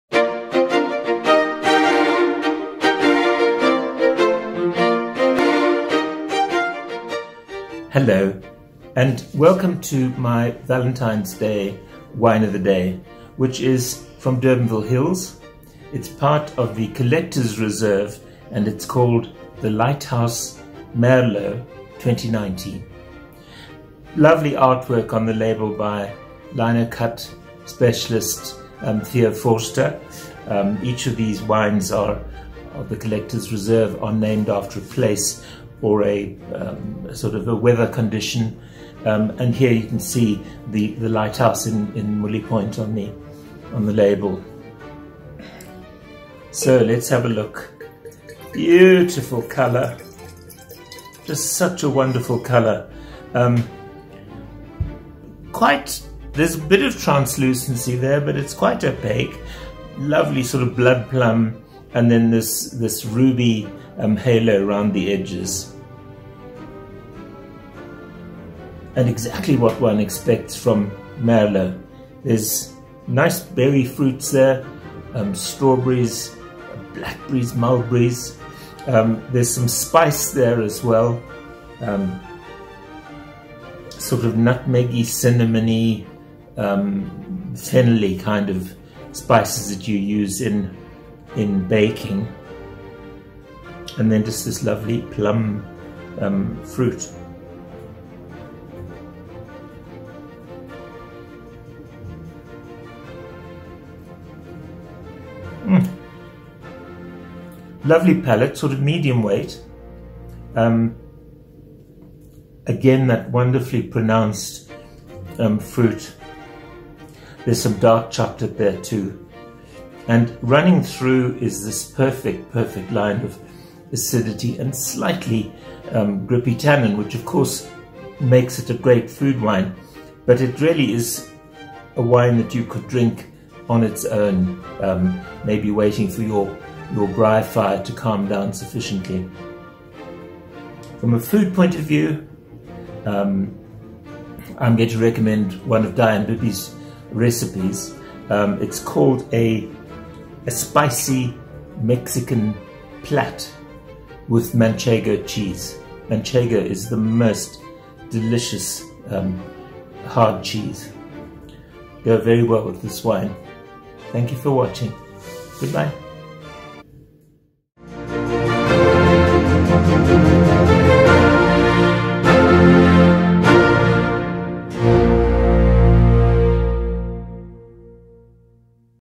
Durbanville Hills Collector's Reserve Merlot 2020
A premium Merlot chosen from some of the finest vineyards in the Durbanville Valley from 9 different wine grape producers. Martin Moore the Cellarmaster is enjoying his 25th vintage as Founding Cellarmaster...